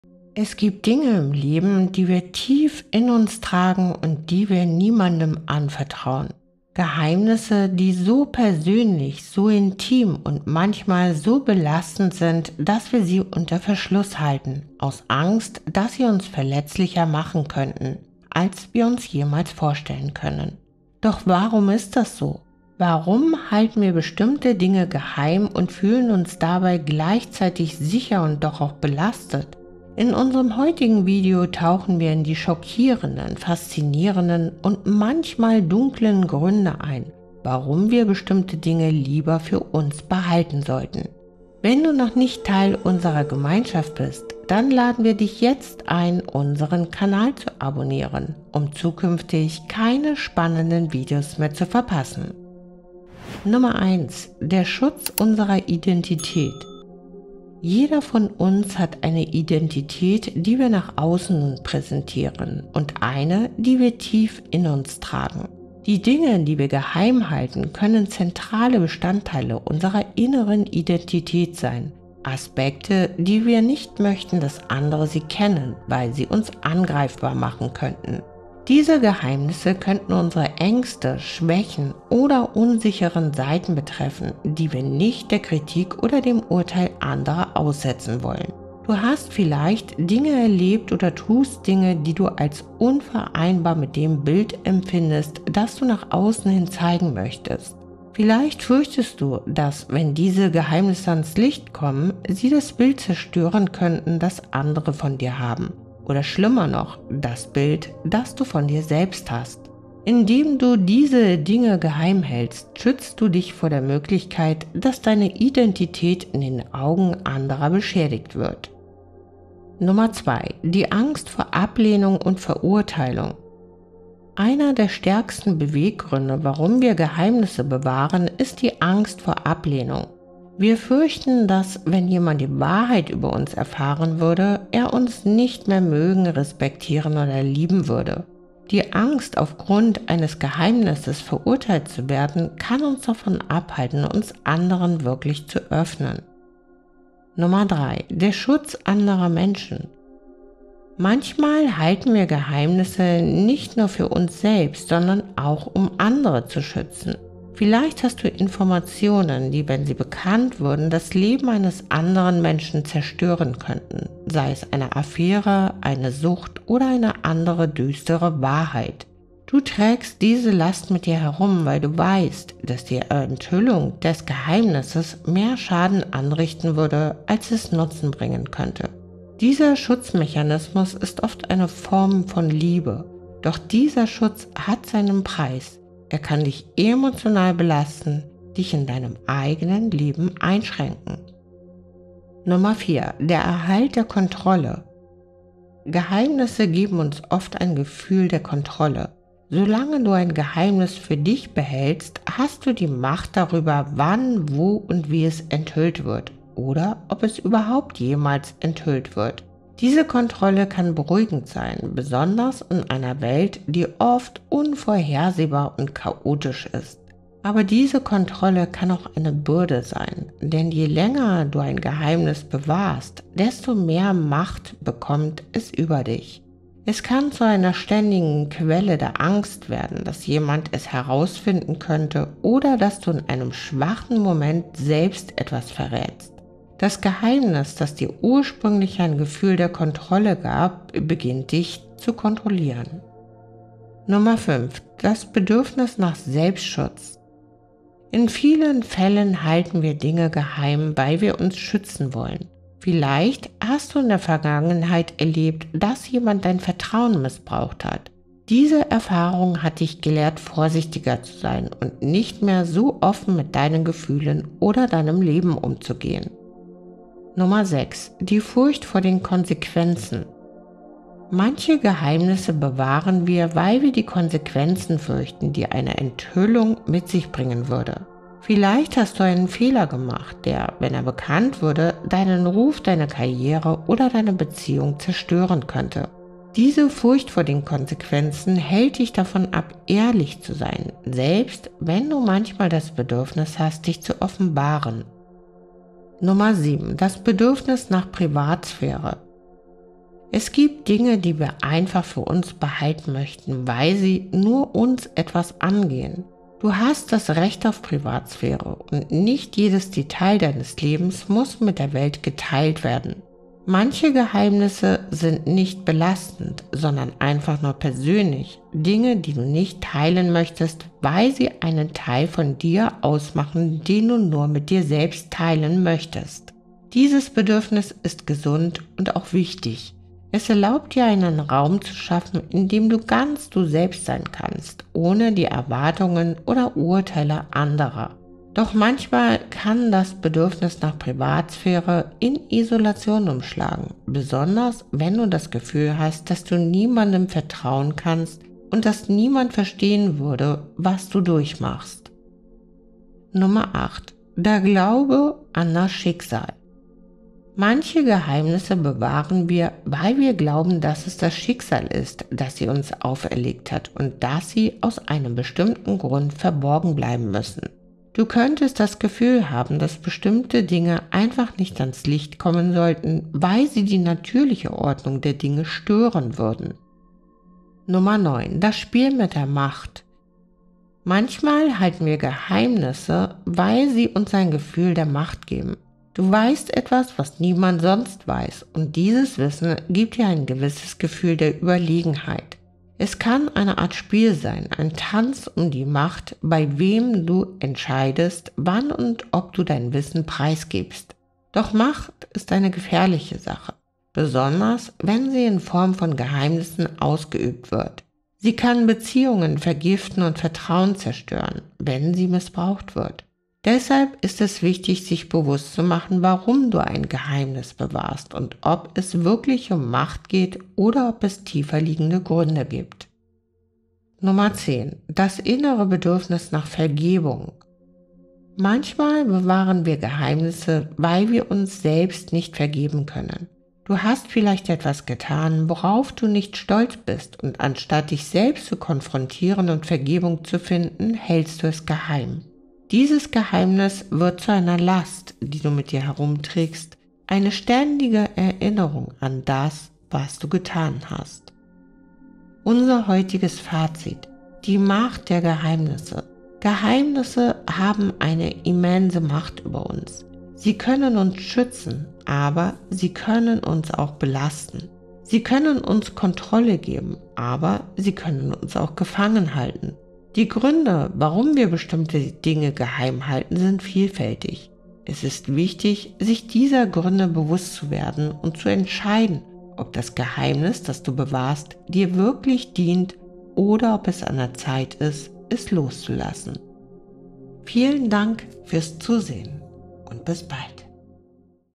Warum wir diese Dinge mit niemandem teilen sollten!
Warum wir Dinge geheim halten und niemandem verraten!

Es gibt Dinge im Leben, die wir tief in uns tragen und die wir niemandem anvertrauen. Geheimnisse, die so persönlich, so intim und manchmal so belastend sind, dass wir sie unter Verschluss halten, aus Angst, dass sie uns verletzlicher machen könnten, als wir uns jemals vorstellen können. Doch warum ist das so? Warum halten wir bestimmte Dinge geheim und fühlen uns dabei gleichzeitig sicher und doch auch belastet? In unserem heutigen Video tauchen wir in die schockierenden, faszinierenden und manchmal dunklen Gründe ein, warum wir bestimmte Dinge lieber für uns behalten sollten.